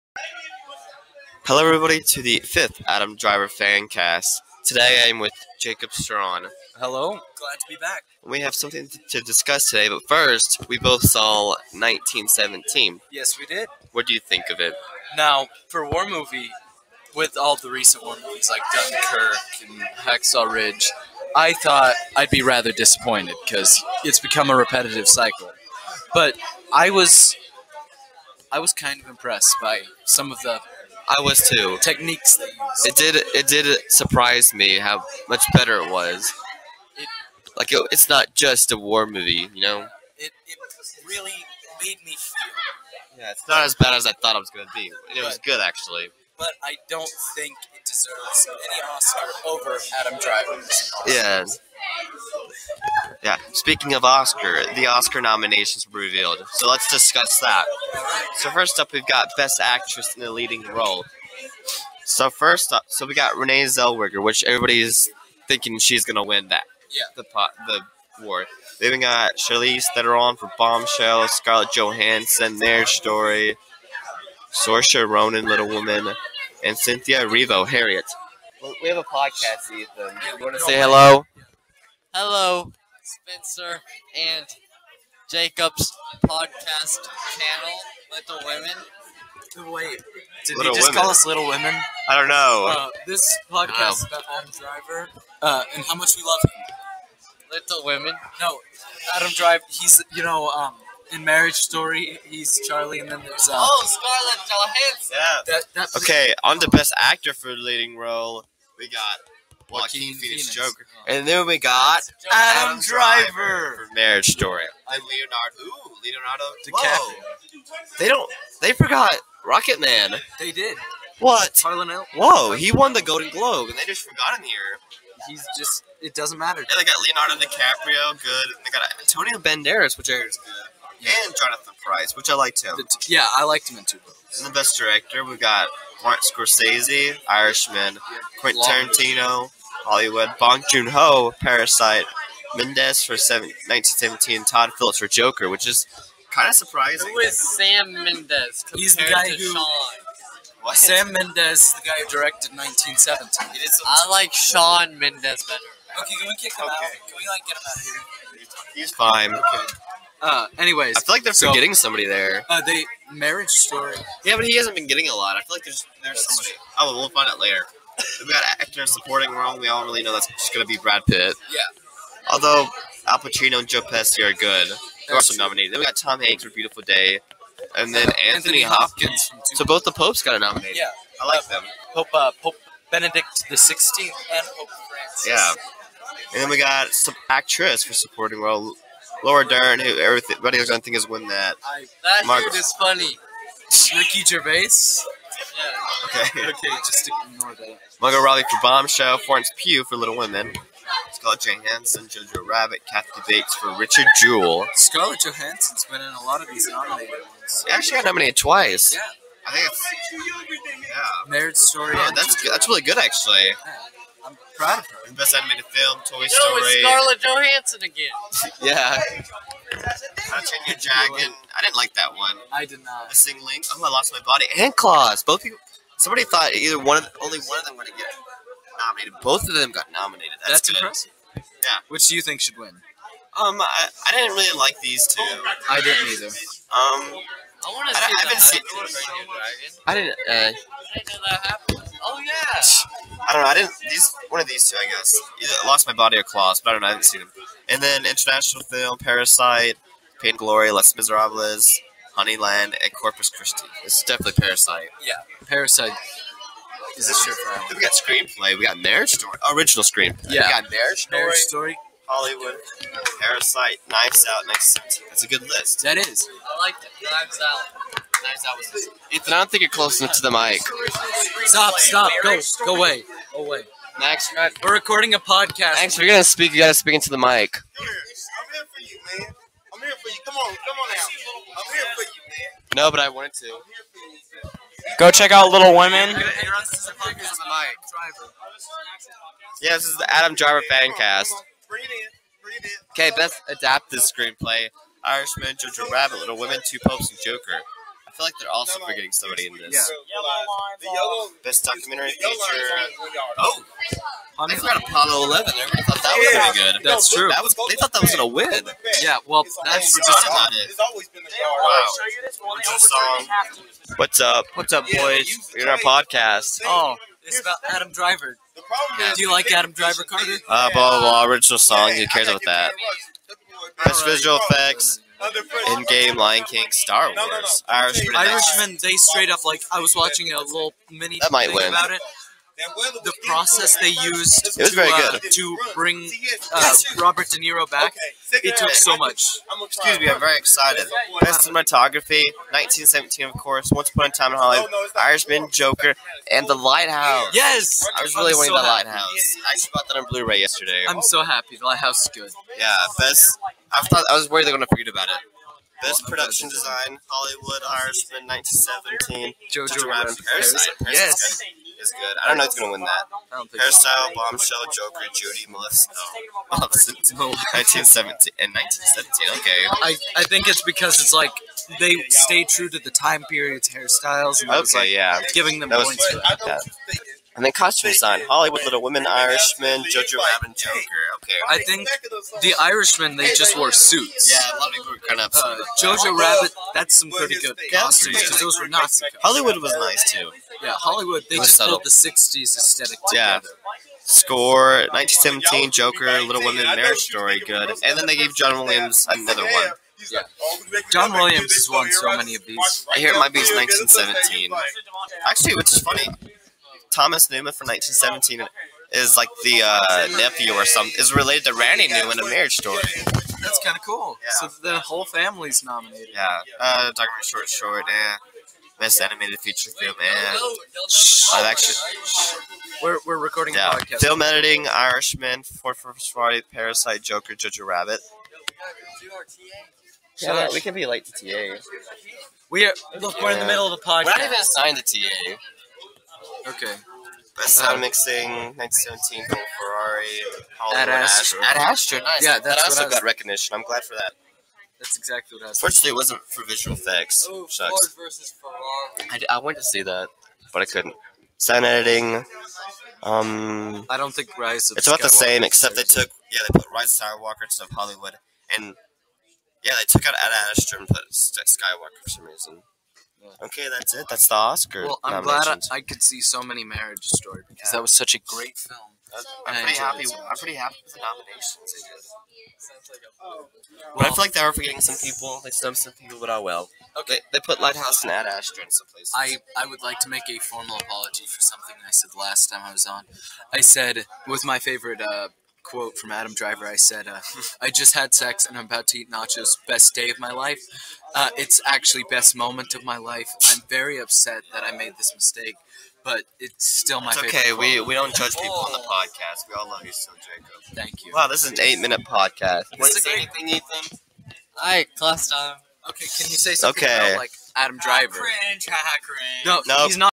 Adam Driver Fan Cast #5 - Oscar Nominees Reactions
In this episode of the podcast, me and Jacob react to the Oscar Nominees and our thought on 1917.